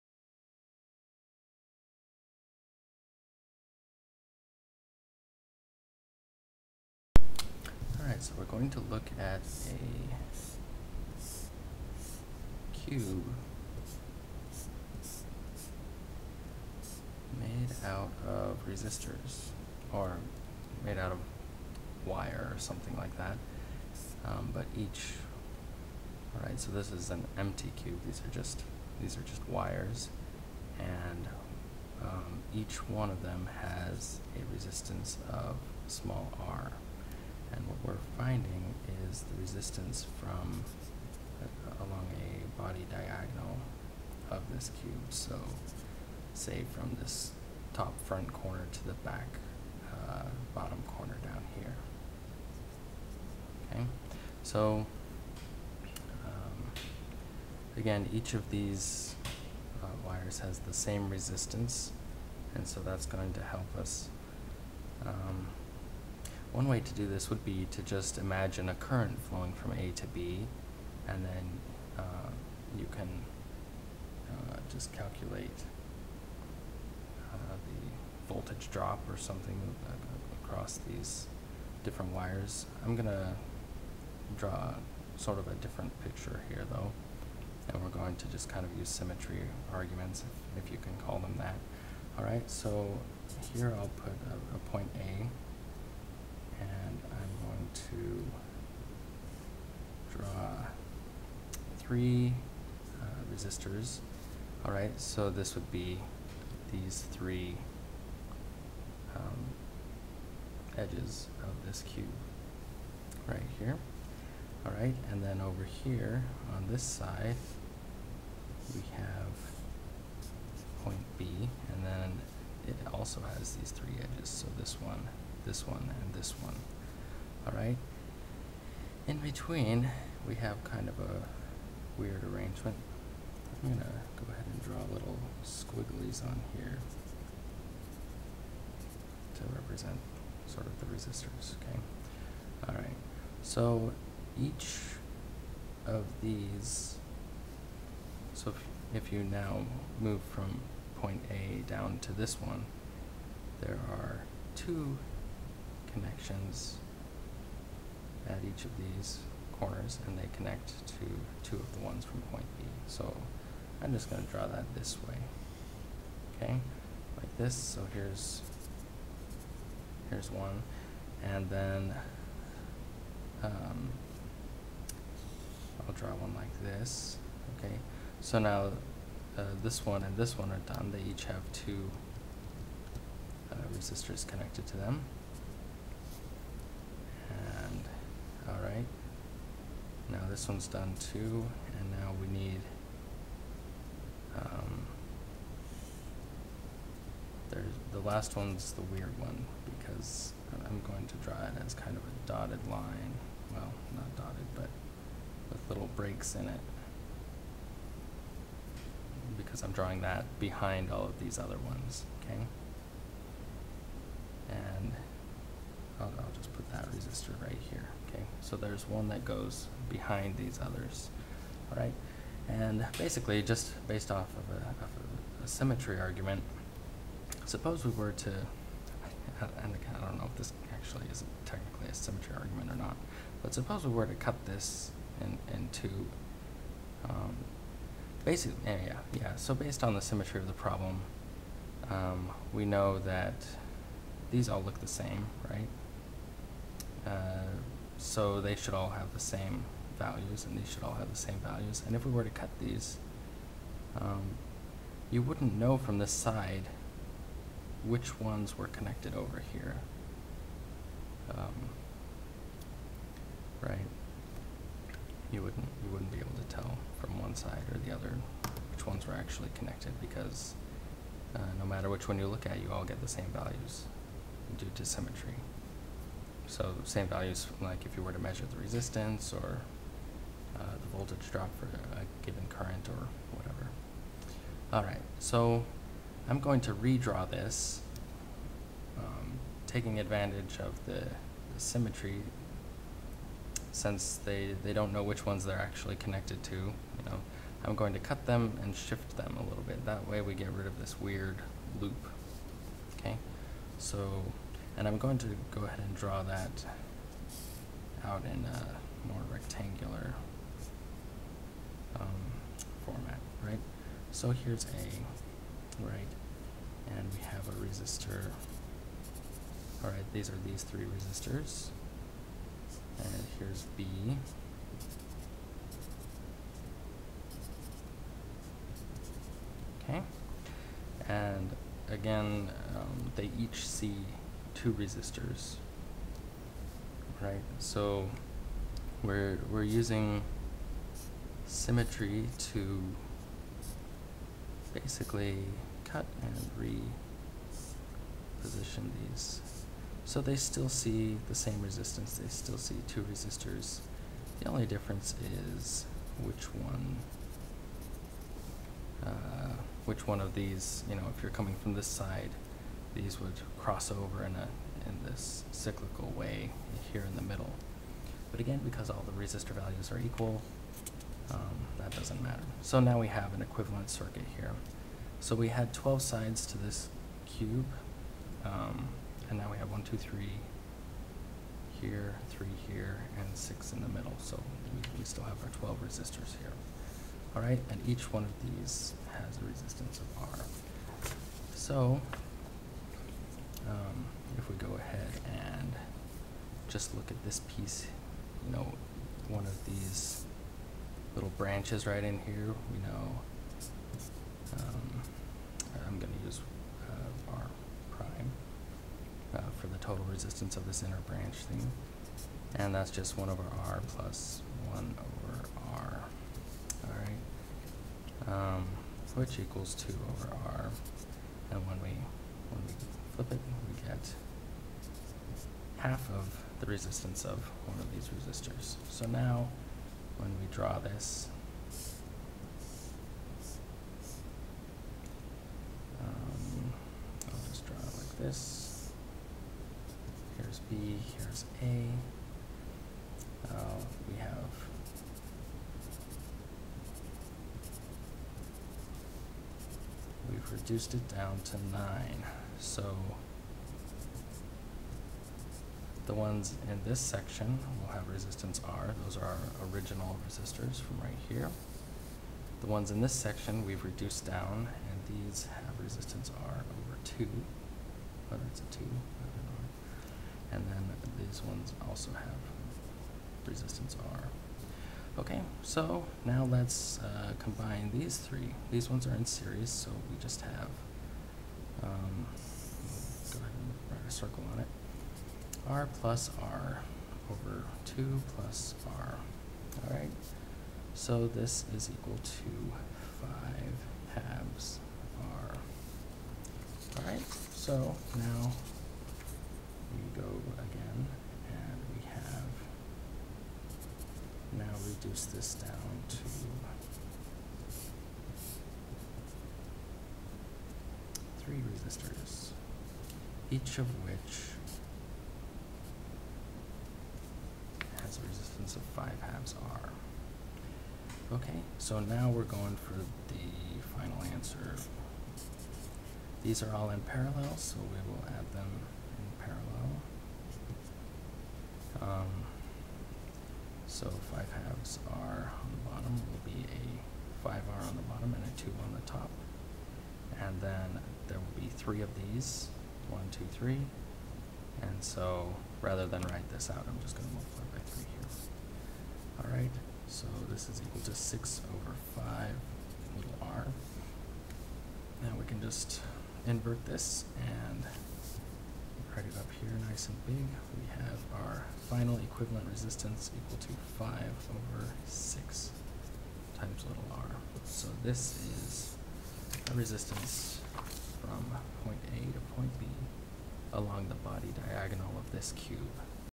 Resistance Across the Body Diagonal of a Resistor Cube
Problem from a Physics PhD Qualifying Exam at Florida State University.